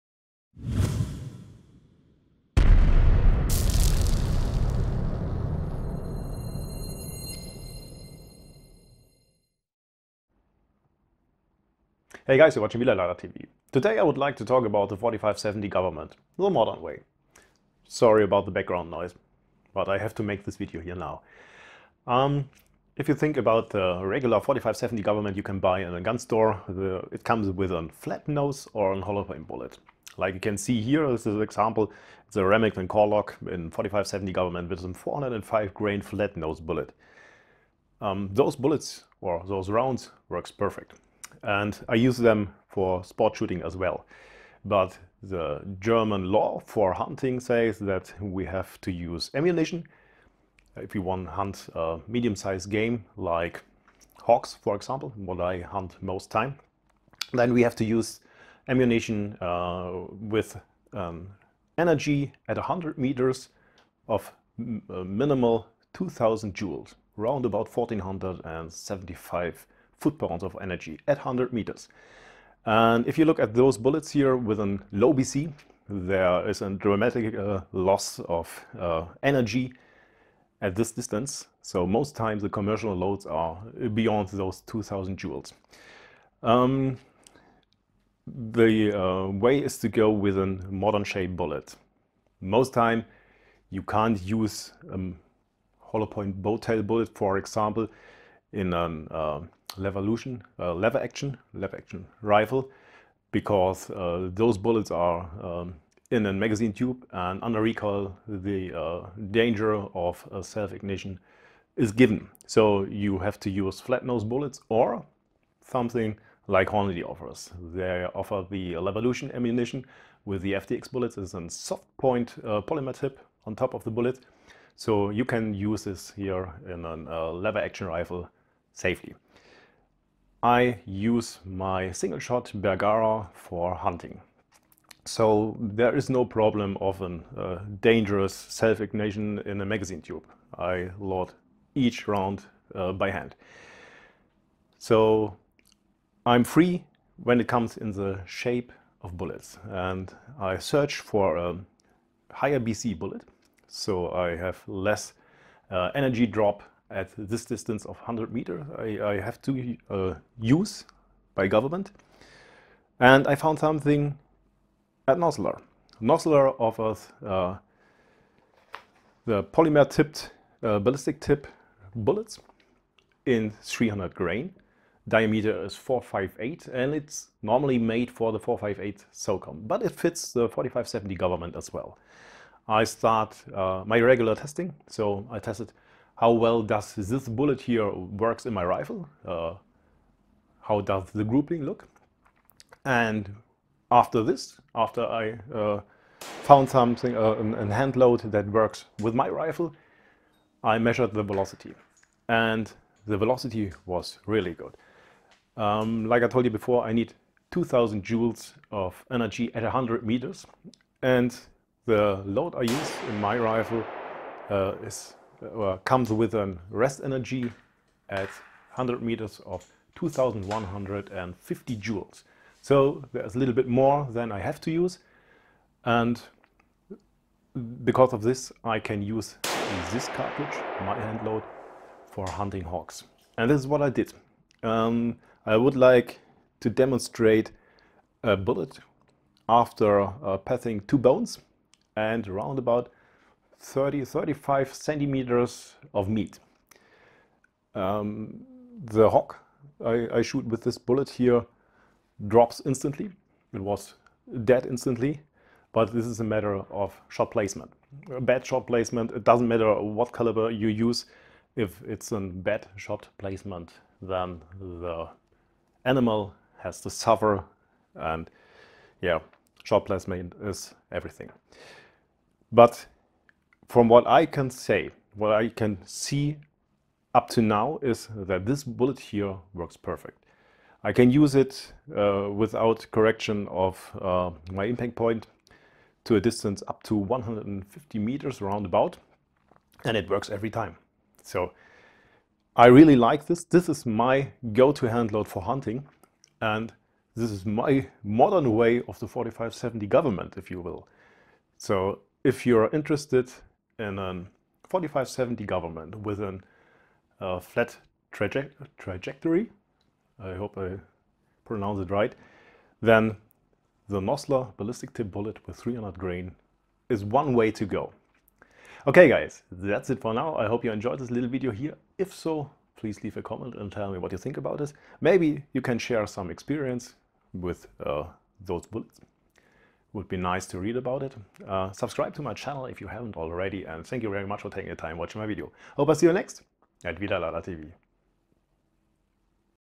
45-70 Government - the modern way with Nosler bullets
In this video I show you my handload in 45-70 Government I use for hunting wild hogs. We have to use ammo that delivers at least 2000 Joule of energy at a distance of 100m. With regular commercial loads in 45-70 this is not given most time. Because of this I create my own modern way of a 45-70 Government with Nosler Ballistic Tip bullets.